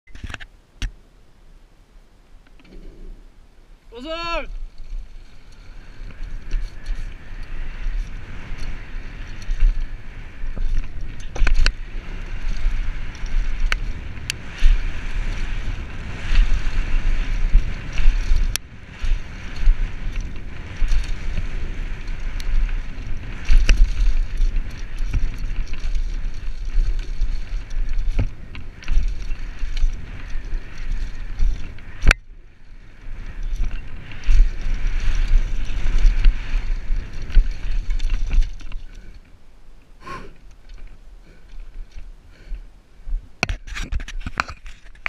Stráne Freeride trail on Hardtail Helmet cam
Recorded on GoPro 3 Black+